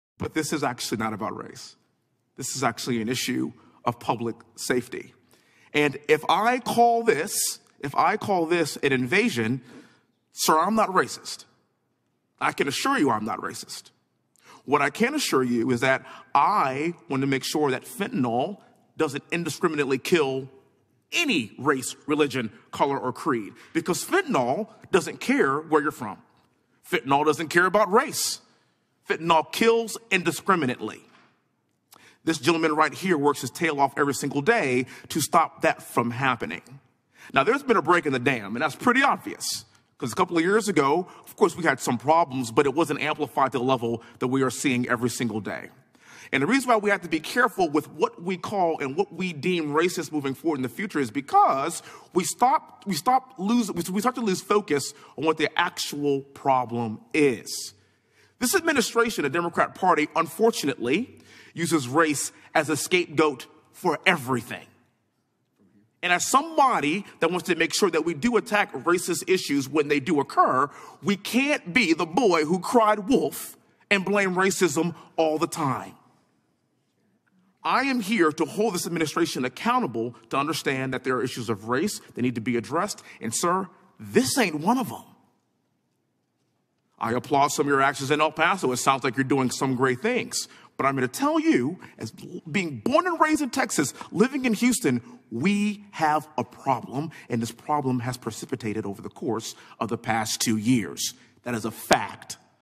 Rep Wesley Hunt Rips Democrats For Crying Racism For Everything
"This administration, the Democrat party unfortunately uses race as a scapegoat for everything"